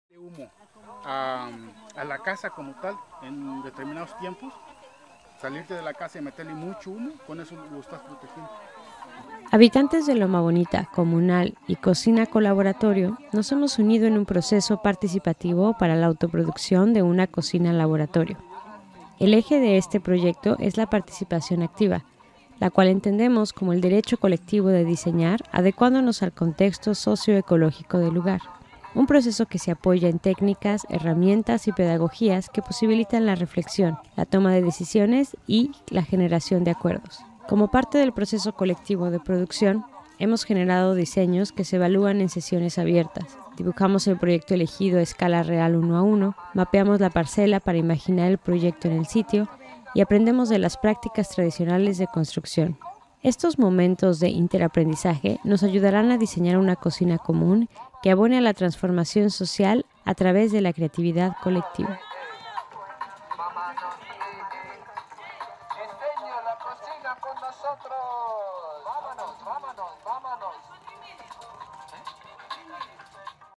Producción Social de una Cocina Parte 3: Gestión y Producción Social
Video para campaña de fondeo para construir la Cocina Laboratorio en Loma Bonita, Chiapas: un espacio de encuentro e innovación, para reconectar a los escosistemas, los sistemas alimentarios y los saberes en la Selva Lacandona.

Video 3: procesos participativos de diseño y gestión.

ENG
Video for the crowdfunding campaign to build a Lab Kitchen in Loma Bonita, Chiapas: a meeting and innovation space, to reconnect ecosystems, food systems and knowledge in the Lacandona Jungle.

Cocina CoLaboratorio + Ejido Loma Bonita + Taller Comunal

Video y edición: Pablo Chavarría Gutiérrez
Sonido: Elizabeth Guerrero y Jesper Buursink